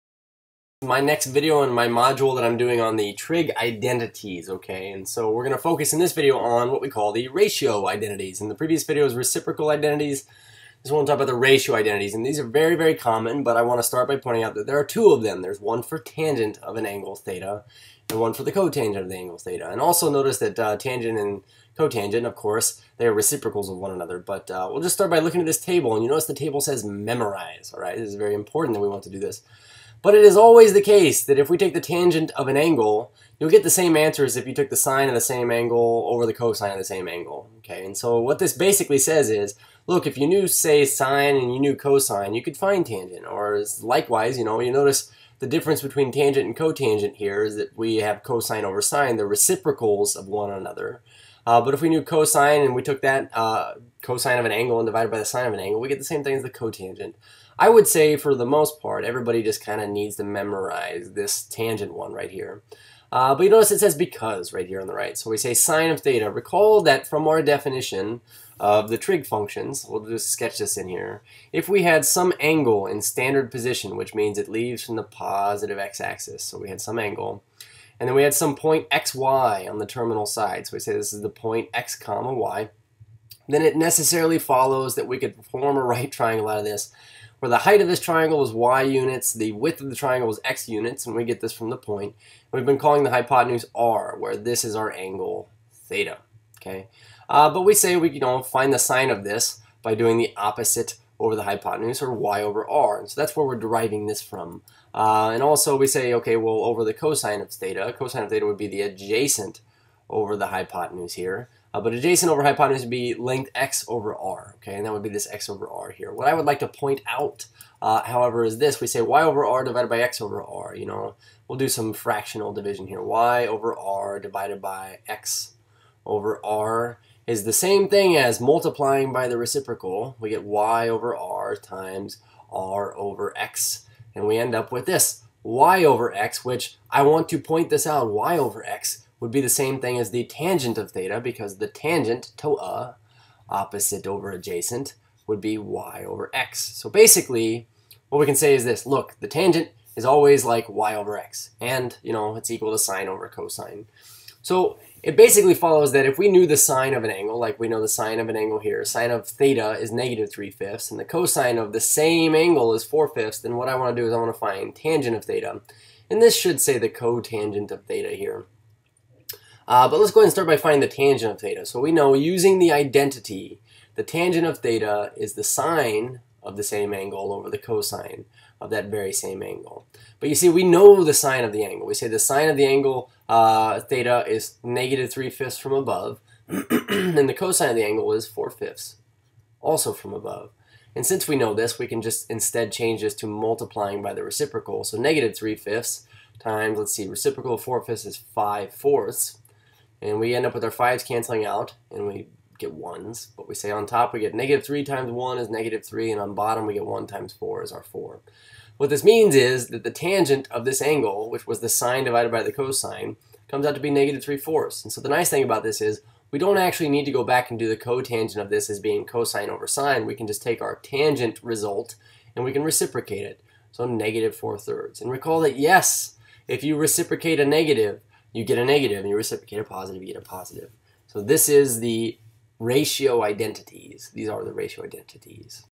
Trig Identities: The Ratio Identities
In this video I introduce the ratio identities.  I show how these may be obtained from the definition of the trig ratios.  I also show how an example of how the ratio identities may be used to find the tangent and cotangent of an angle when both the sine and cosine of the angle are known/given.